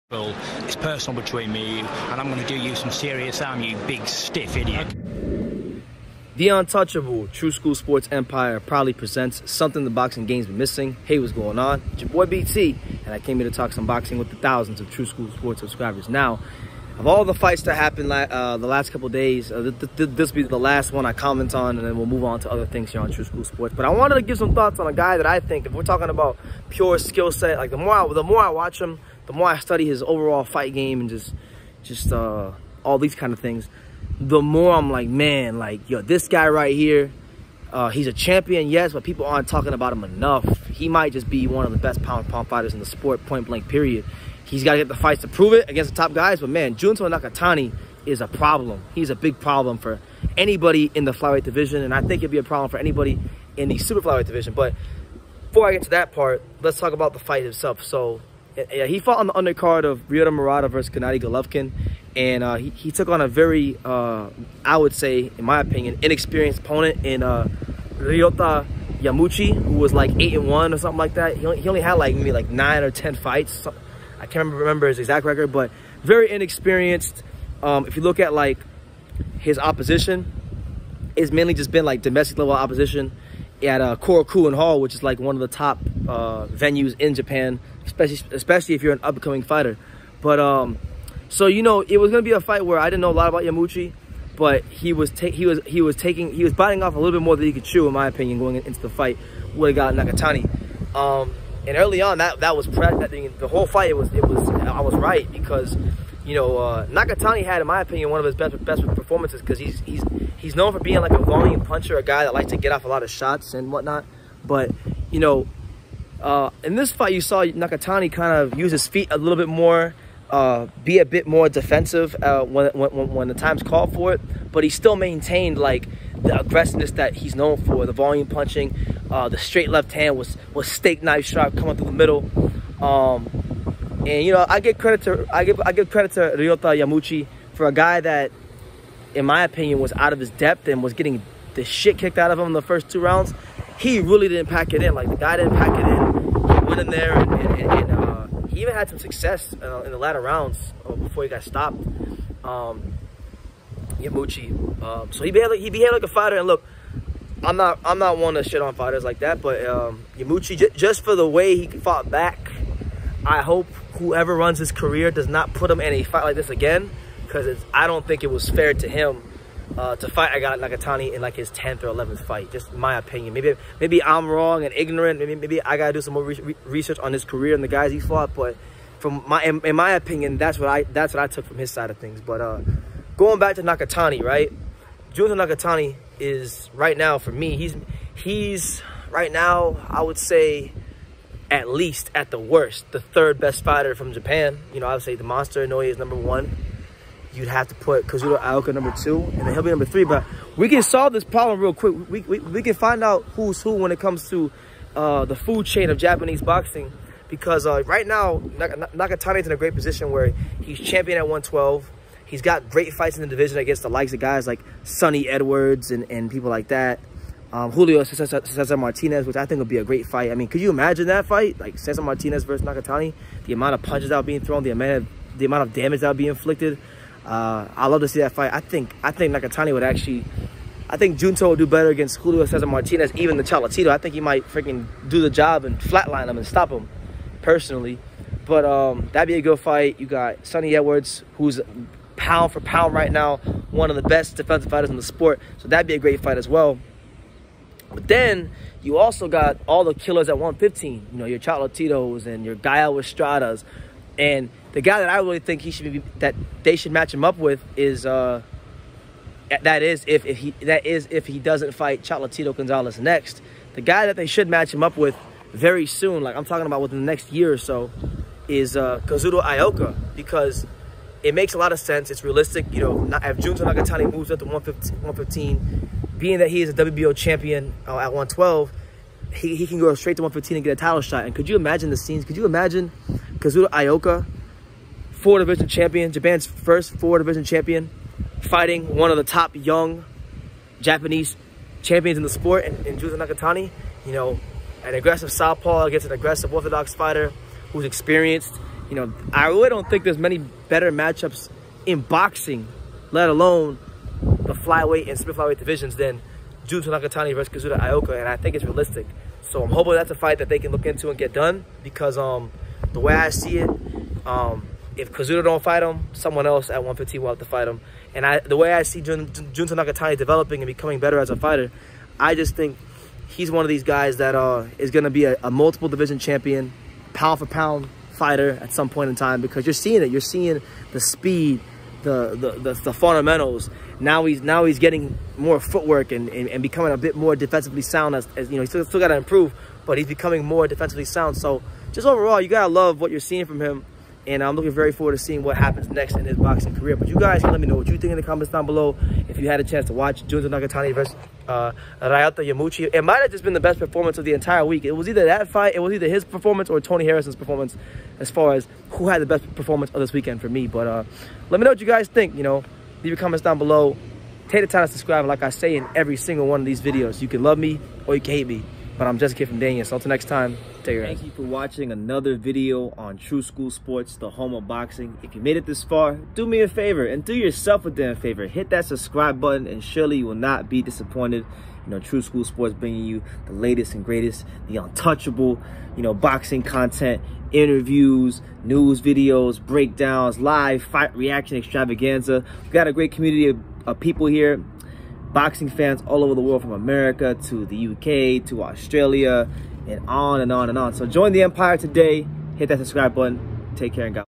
JUNTO NAKATANI FUTURE P4P STAR WOWS AGAIN VS YAMAUCHI; FUTURE JAPAN SUPER FIGHT VS KAZUTO IOKA? 🇯🇵
Junto Nakatani is the real deal